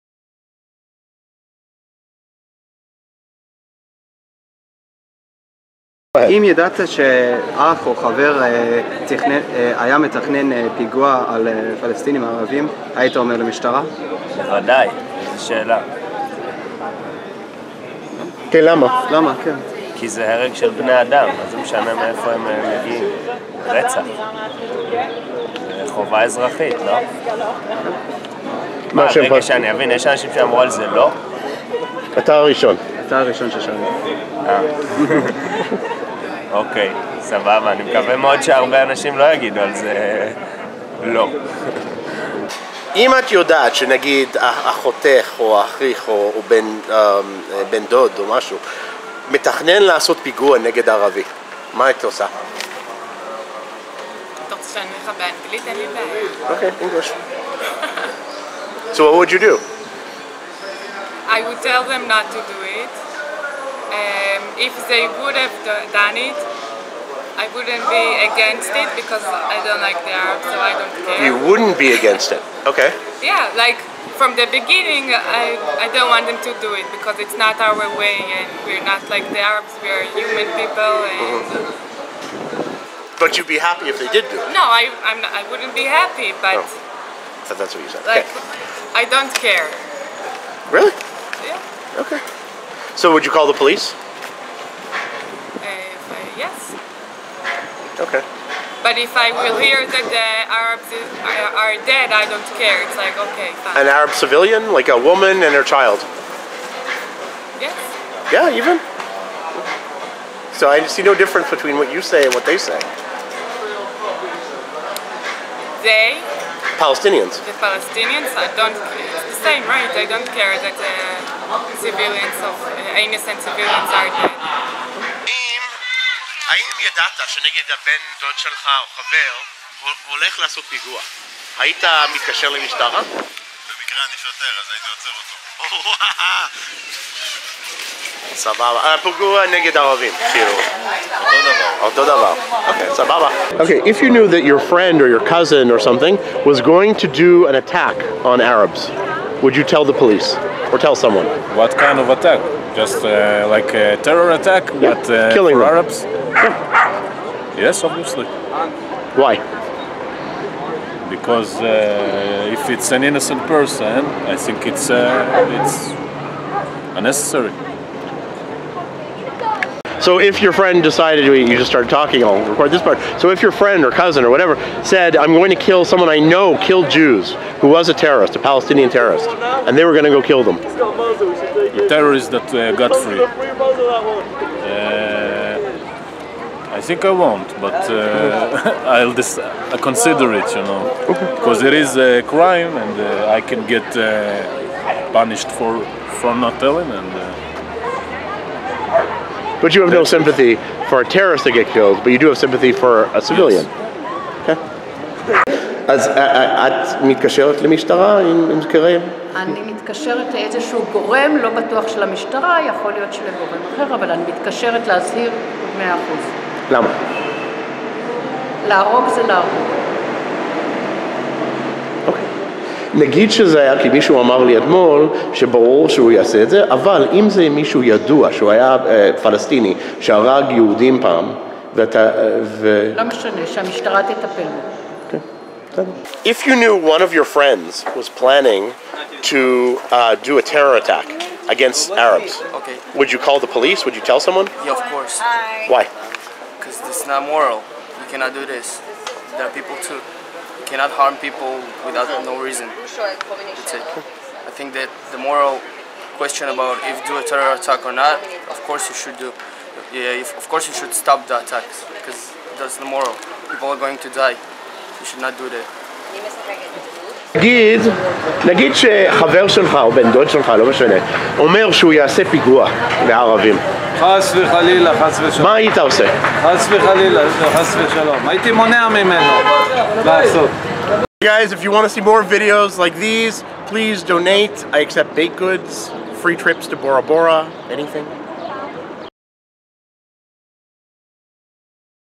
Israelis: Would you call the police on someone planning to attack Palestinians?
This project is made possible by donations from viewers like you.
Want to know what Israelis and Palestinians in the Middle East really think about the conflict? Ask a question and I will get answers.